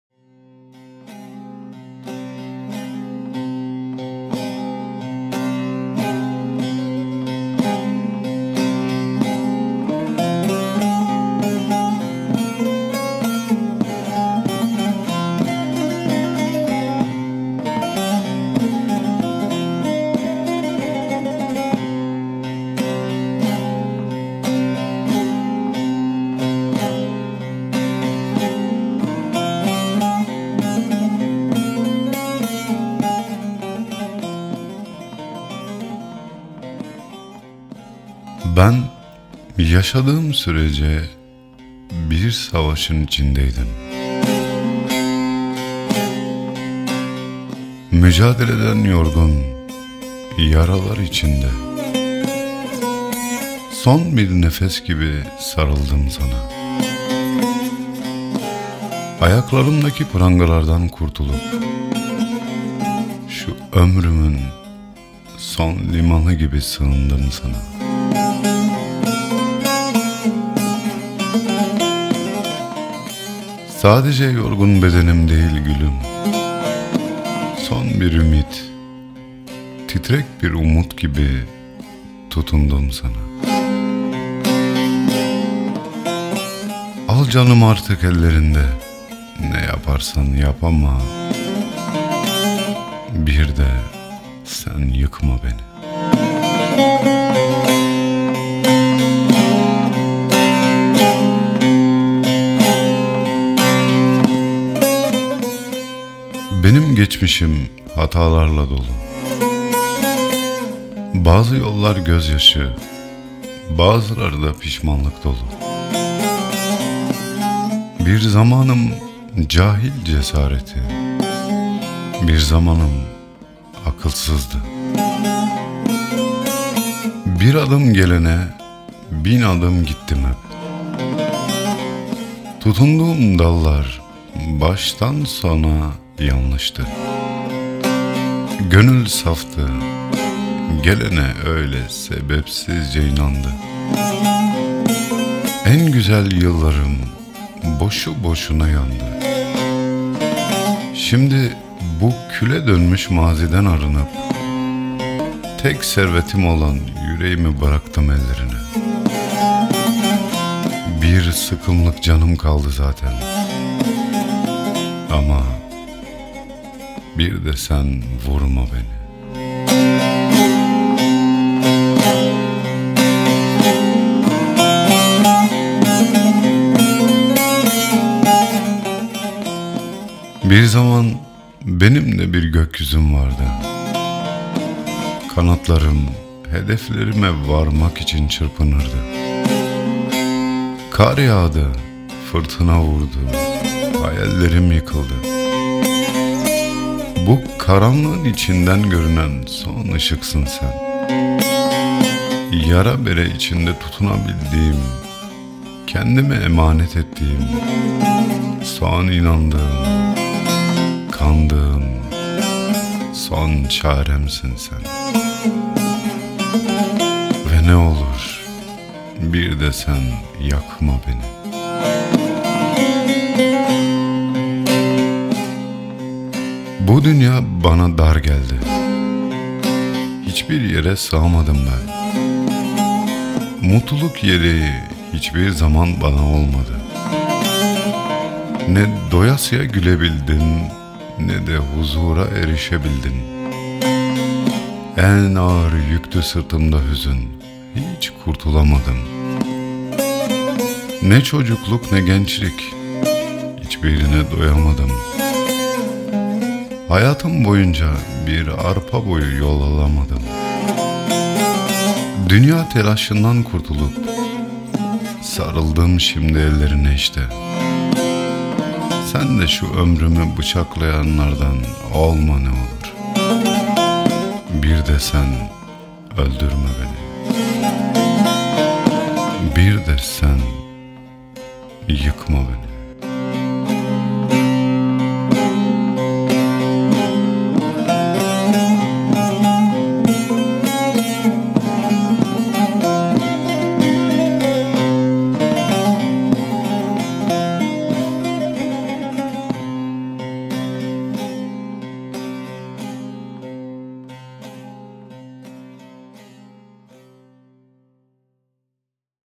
Birde sen Yıkma Beni (Şiir) Yaman Karaca
Ben yaşadığım sürece bir savaşın içindeydim
Mücadeleden yorgun yaralar içinde
Son bir nefes gibi sarıldım sana
Ayaklarımda ki prangalardan kurtulup
Ömrümün son limanı gibi sığındım sana
Sadece yorgun bedenim değil gülüm
Son bir ümit, titrek bir umut gibi tutundum sana
Al canım artık ellerinde ne yaparsan yap ama
Bir de sen yıkma…

Benim geçmişim  hatalarla dolu
Bazı yollar göz yaşı bazılarıda pişmanlık dolu
Bir zamanım cahil cesareti  bir zamanım akılsızdı
Bir adım gelene bin adım gittim hep
Tutunduğum dallar  baştan sona yanlıştı
Gönül saftı gelene öyle sebepsiz inandı
En güzel yıllarım boşu boşuna yandı
Şimdi bu küle dönmüş maziden arınıp
Tek servetim olan yüreğimi bıraktım ellerine
Bir sıkımlık canım kaldı zaten benim
Bir de sen vurma beni…

Bir zaman benimde bir gök yüzüm vardı
Kanatlarım hedeflerime varmak çırpınırdı
Kar yağdı fırtına  vurdu hayallerim yıkıldı
Bu karanlığın içinden görünen son ışıksın sen
Yara bere içinde tutunabilidiğim kendimi emanet ettiğim
Son inandığım kandığım son çaremsin sen
Bir de sen yakma beni…

Bu dünya bana dar geldi
Hiç bir yere sığamadım ben
Mutluluk yeleği hiç bir zaman bana olmadı
Ne doyasıya gülebildim ne de huzura erişebildim
En ağır yüktü sırtımda hüzün hiç kurtulamadım
Ne çoçukluk ne gençlik hiç birine doyamadım
Hayatım boyunca bir arpa boyu yol alamadım
Dünya telaşından kurtulup sarıldım şimdi ellerine işte
Sende şu ömrümü bıcaklayanlardan olma ne olur
Bir de sen öldürme beni…
Yaman Karaca

Ayrıca Mucizem kitabımız da sitemizde  satışa başlamıştır...

Şiir : @yamansiir
Seslendirme :@yamansiir
İnstagram hesabımız : @yamansiir
tiktok hesabımız : @yamansiir_